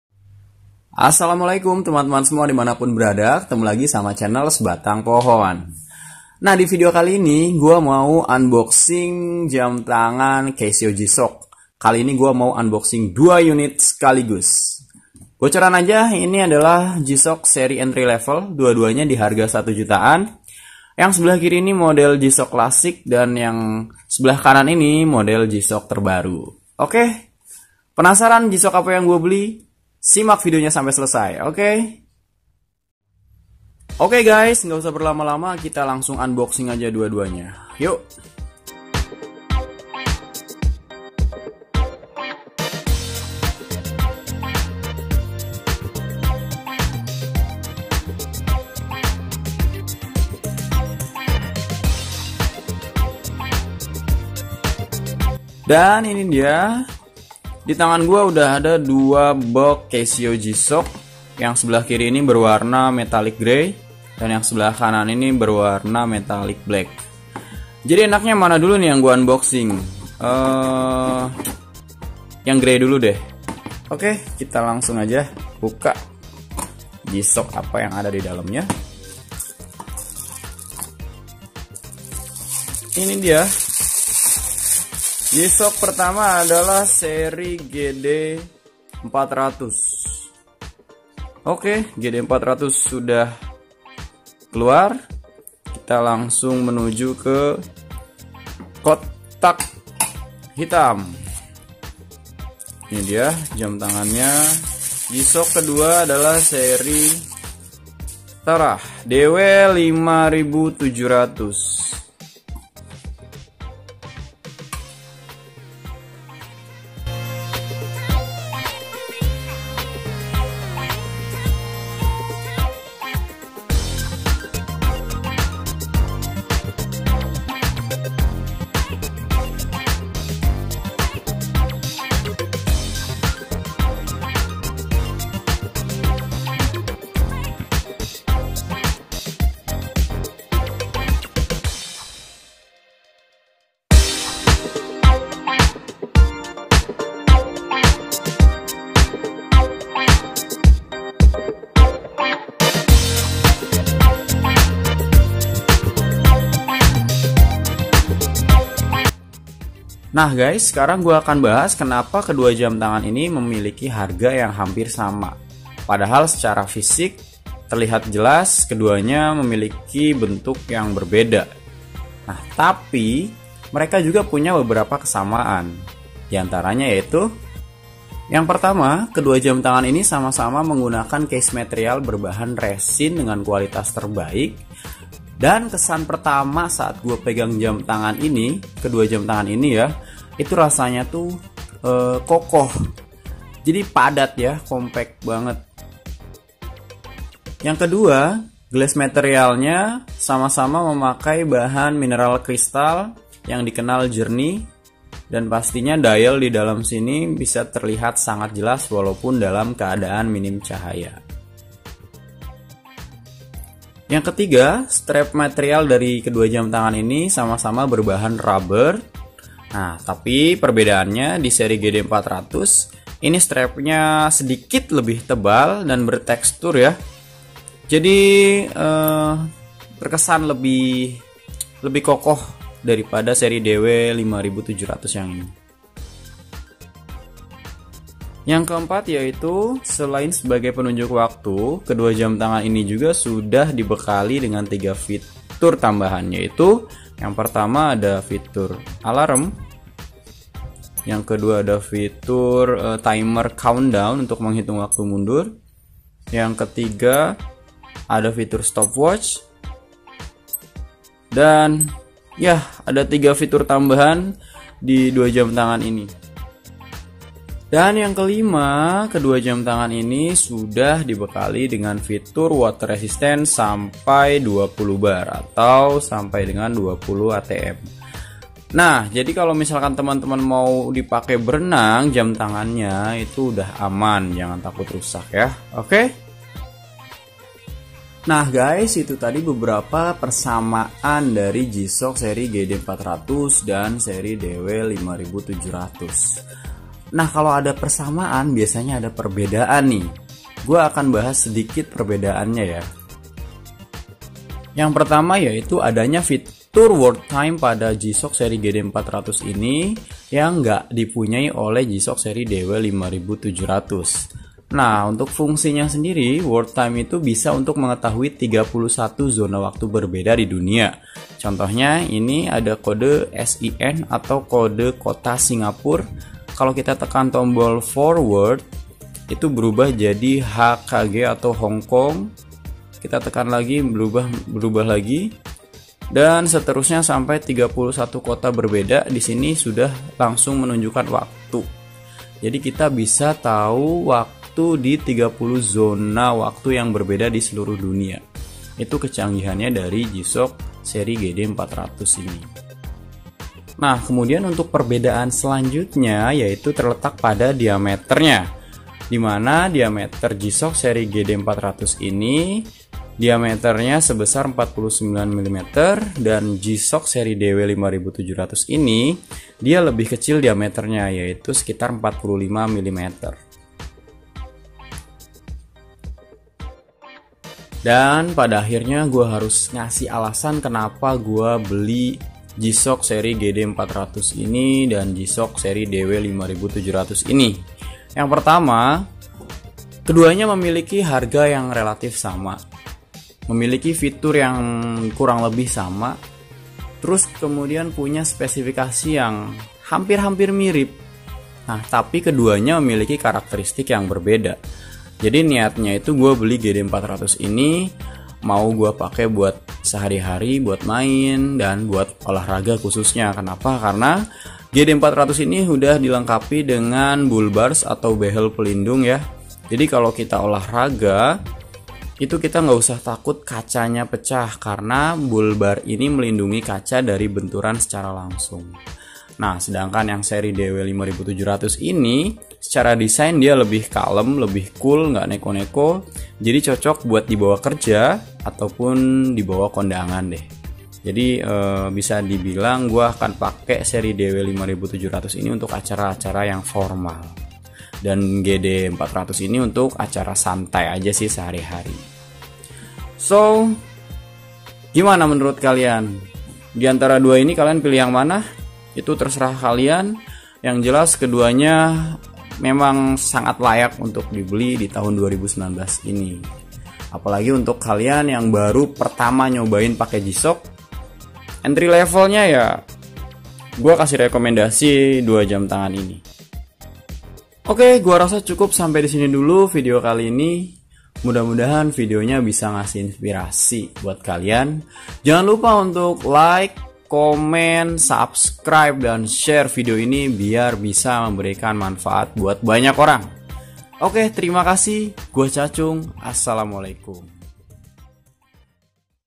G-Shock harga 1 Jutaan‼️ Wajib beli‼️
Wajib beli nih gais. !!!

G-Shock cuma 1 jutaan aja udah dapet kualitas terbaik 👍💯

Buat yang tanya belinya dimana?
nih tinggal klik aja link di bawah